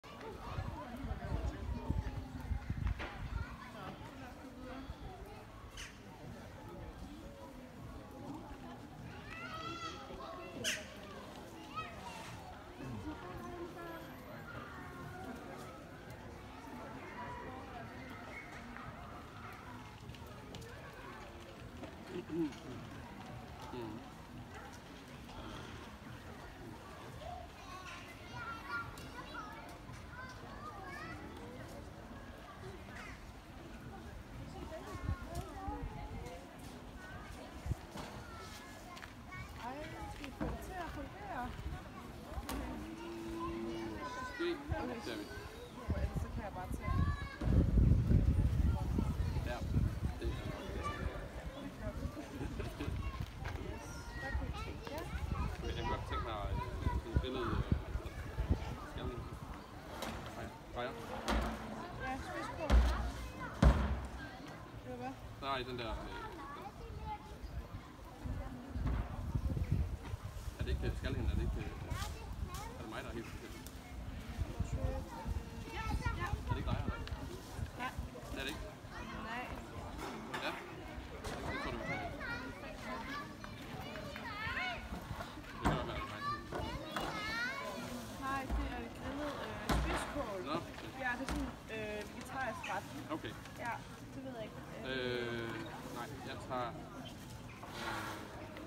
Seeing a Family of Asian elephants in the Copenhagen zoo
I made this video Back in the Copenhagen zoo of course as you can see in the video you can see a family of Asian elephants They are Highly intelligent and Amazing creatures That can be found in Asia